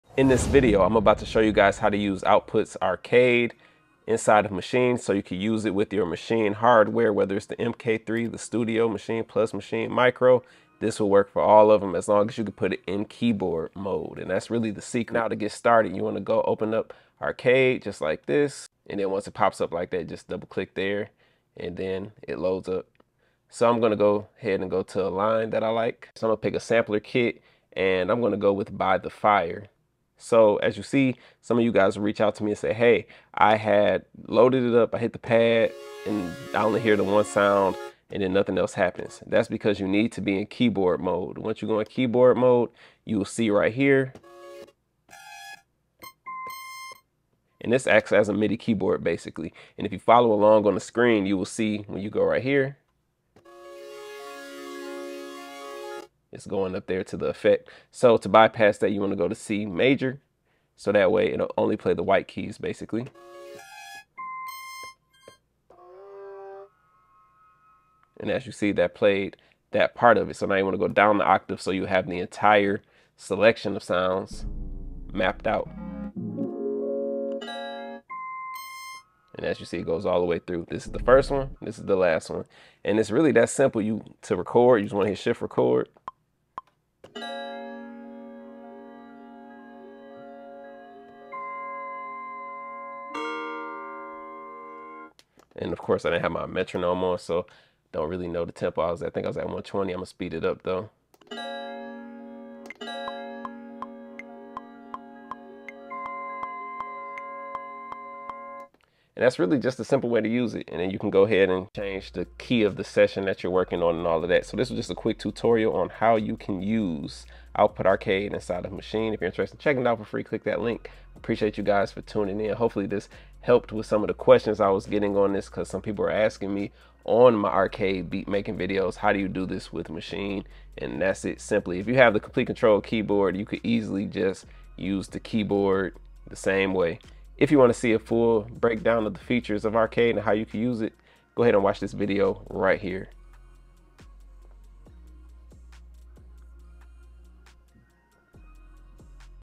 How To Use Output Arcade In Maschine - The Ultimate Tutorial!
In this video tutorial, I will show you how to use Output Arcade VST in Maschine! So you can make beats using Native Instruments Maschine with Arcade by Output!

Upload your music to streaming services such as Spotify, Apple Music & More

The Camera I use
The Mic
Studio Monitors
Audio Interface